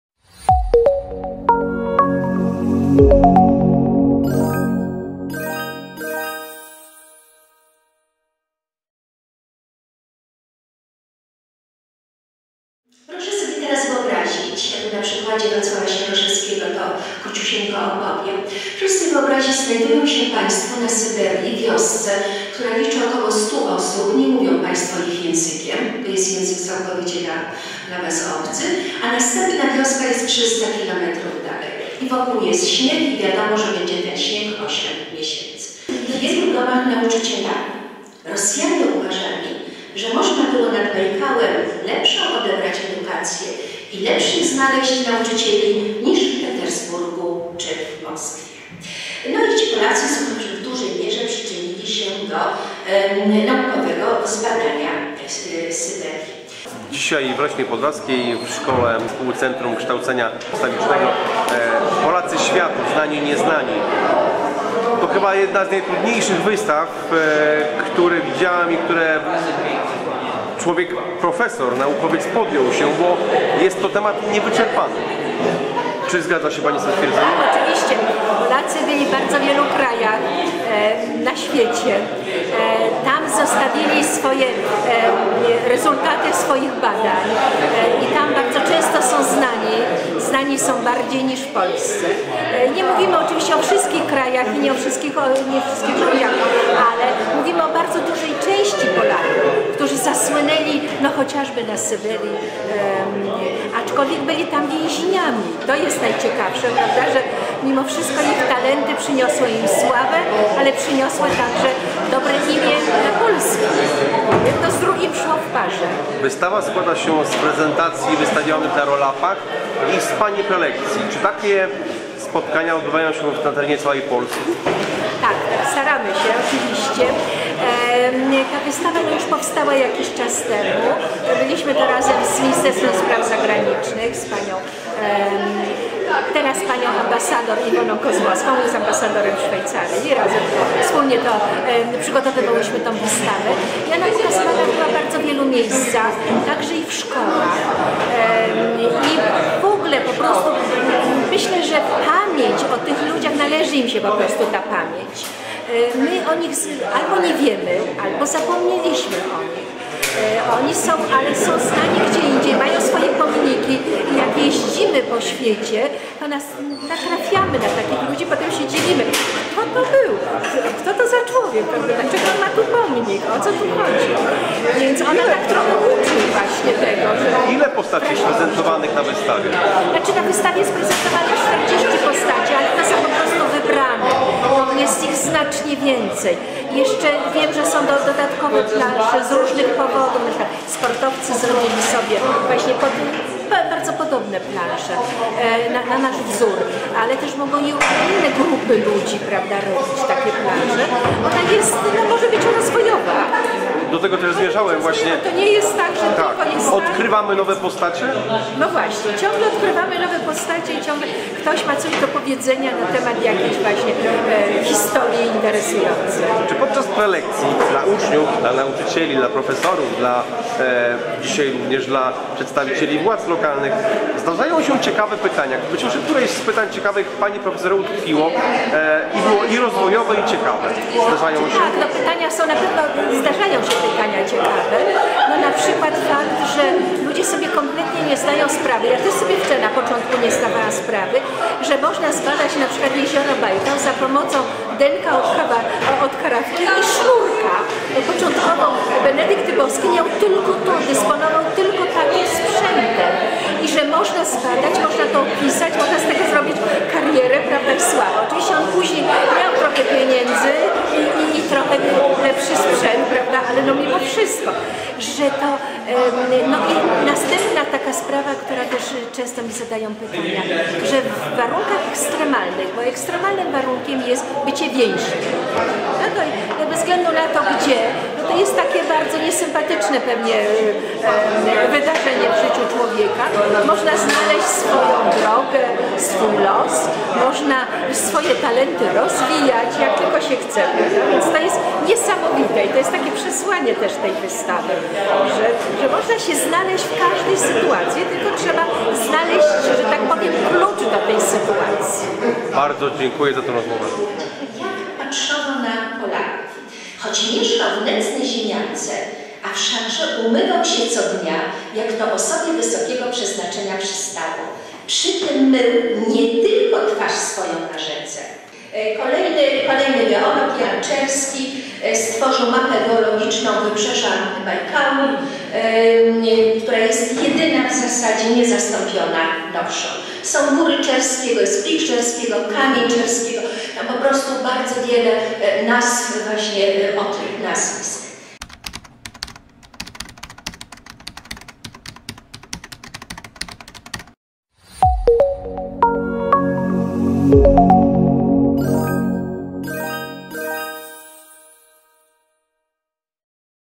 Wystawa „Znani nieznani” w ZSCKR w Leśnej Podlaskiej
W Zespole Szkół Centrum Kształcenia Rolniczego w Leśnej Podlaskiej odbyło się spotkanie prelekcyjne związane z wystawą "Znani nieznani" prezentującą dokonania 44 wybitnych Polaków, dla wielu nieznanych lub zapomnianych. To niezwykle cenna lekcja historii wchodząca w skład cyklu przedsięwzięć zatytułowanych „Polacy światu”, realizowanych przez Ministerstwo Spraw Zagranicznych we współpracy z Archiwum Polskiej Akademii Nauk.